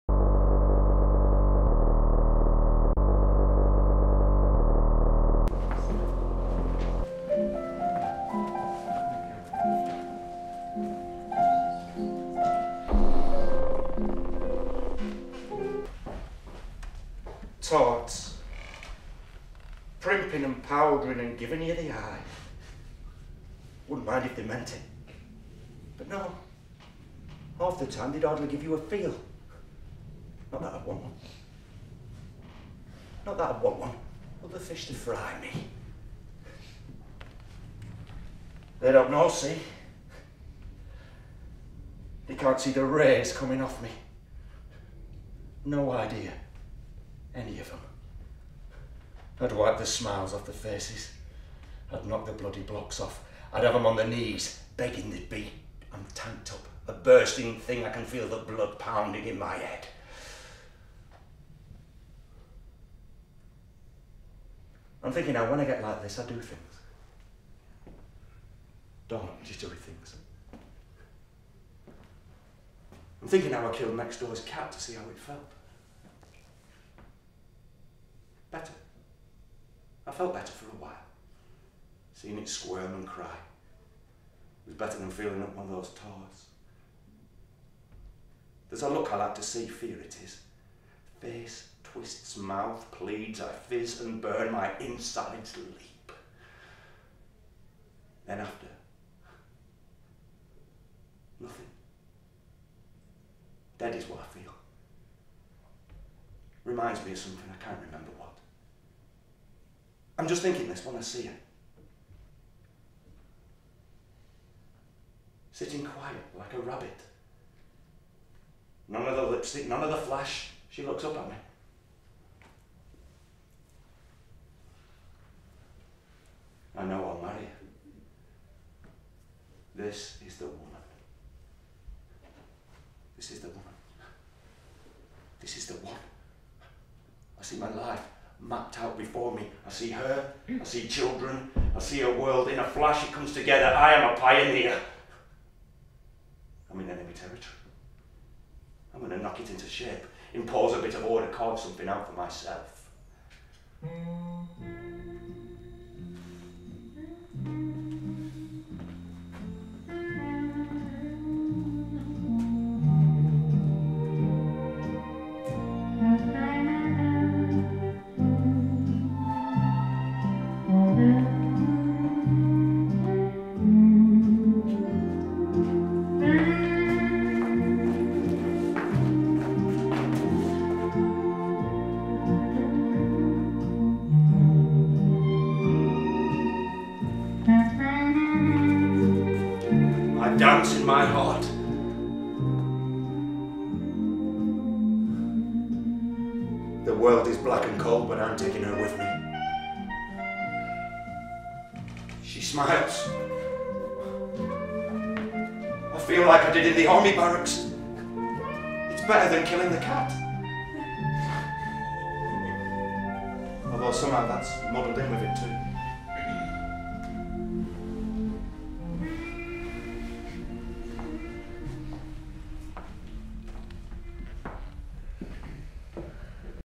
Zach Lee as Billy "Killing the Cat" speech from FIVE KINDS OF SILENCE
"It requires particular courage of an actor to play the kind of  unreconstructed psychopath we meet in Billy - Zach Lee more than rises to the challenge. His performance is pure lean, musclebound menace, shot through with an equally disturbing seam of vulnerability" (Time Out)

"As Billy, the father, Zach Lee is stunning, making it perfectly credible that the three women could loathe and love him at the same time; his character is both a perpetrator and a victim of violence, suffering from alcoholism and epilepsy, yet, however monstrously he behaves, the actor is still able to eke out a degree of sympathy for him" (The Public Reviews)

"Zach Lee as Billy can change from beautiful to monstrous in a moment and is always charismatic" (Plays International)

"Billy, played with terrifying ferocity by Zach Lee" (Whats On Stage)

"Zach Lee is outstanding as Billy, his is a dervish of a performance. At points he is squirming and crawling around the floor like Gollum, literally spitting out his recollections and daydreams with visceral intensity, and at others he lurks in the shadows of the stage like an aphotic, undead sentinal" (Nampam)

FIVE KINDS OF SILENCE by Shelagh Stephenson
Directed by Chris Loveless for Stepping Out Theatre
Jan 29th - Feb 17th 2013 White bear, London.